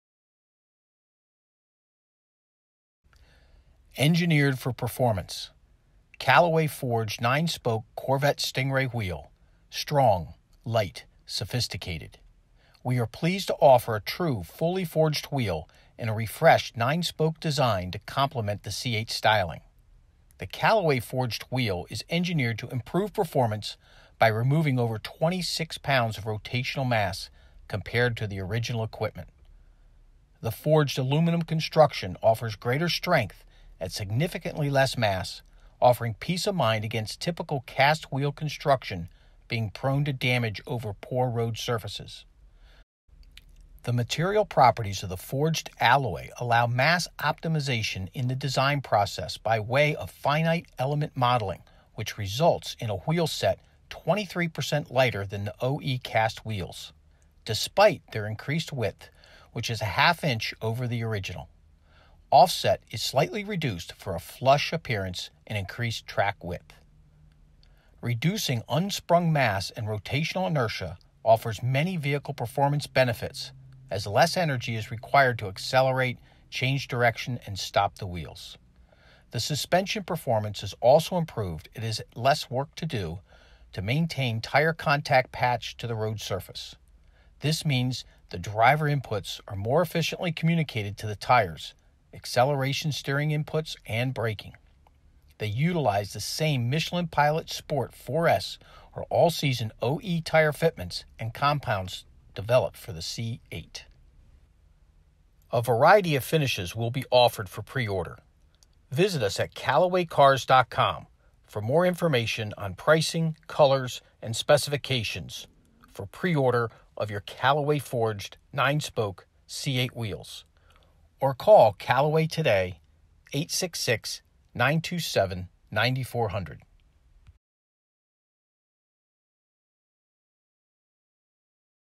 Callaway Forged C8 Corvette Wheels 2020 2021 Lighter Stronger Wider Available Now For Pre-Order
Fully-forged aluminum wheelset that is 26 pounds lighter than OEM stock wheels (total) while being a half-inch wider reducing unsprung mass and rotational inertia. The resulting wider track allows for an improved stance and aggressive look.
The best performance improvement you can make to your 2020 or 2021 Corvette coupe or convertible right now.

Available in a variety of color finishes as shown in Ceramic Polished with clearcoat finish.  Also available in Matte Bronze, Satin Black, Gloss Titanium and Black Chrome PVD.